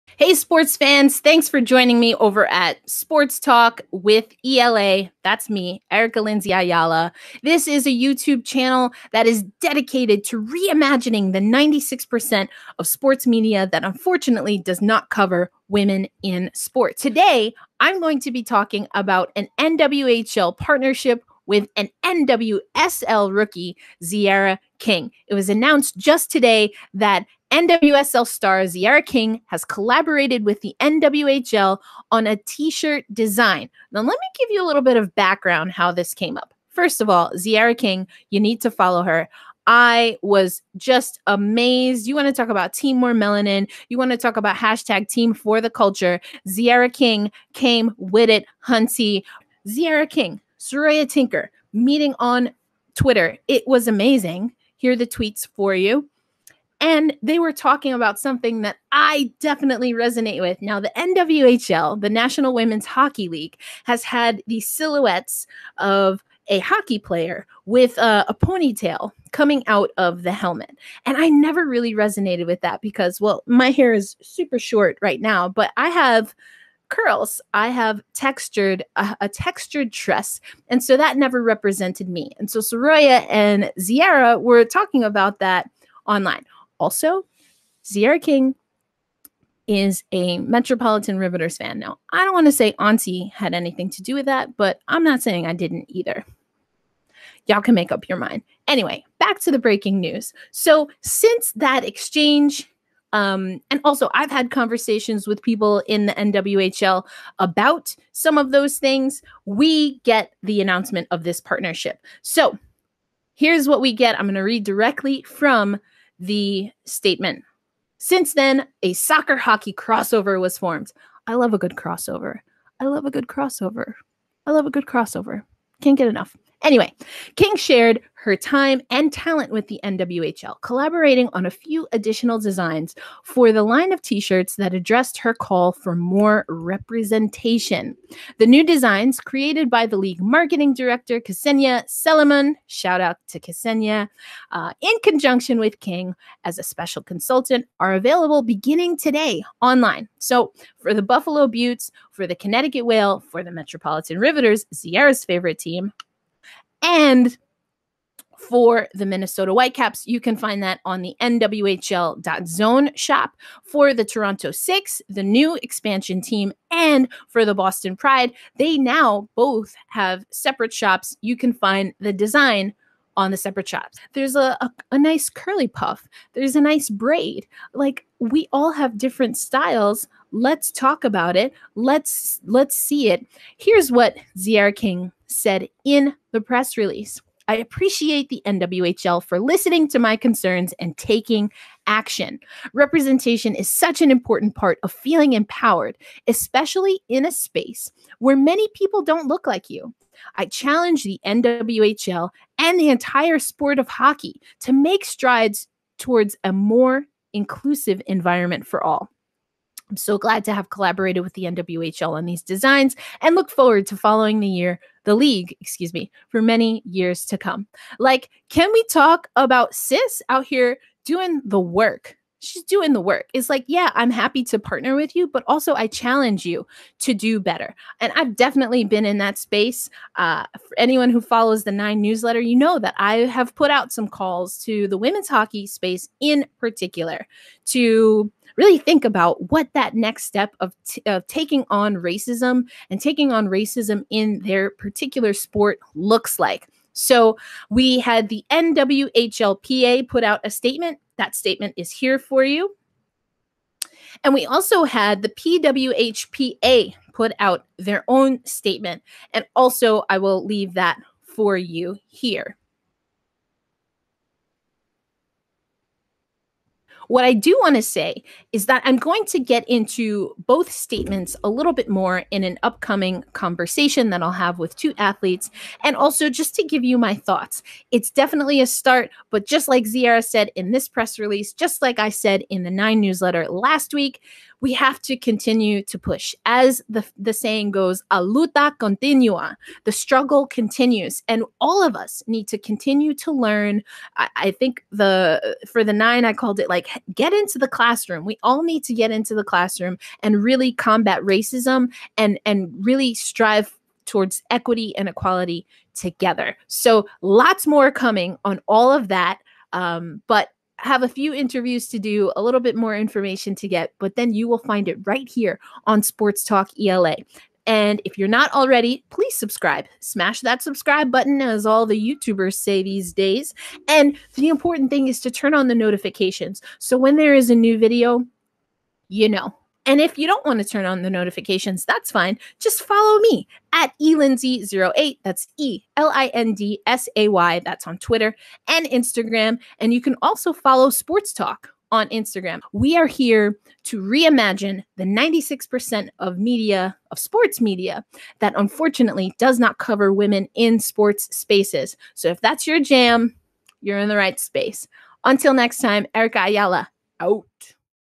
Utah Royals rookie Tziarra King x NWHL partner for new line of women's hockey t-shirts
Today the National Women's Hockey League announced a special line of apparel created in conjunction with special consultant Tziarra King of the Utah Royals.

Does this mean the NWHL is one step closer to Black Rosie? Watch my video on changing the Metropolitan Riveters logo: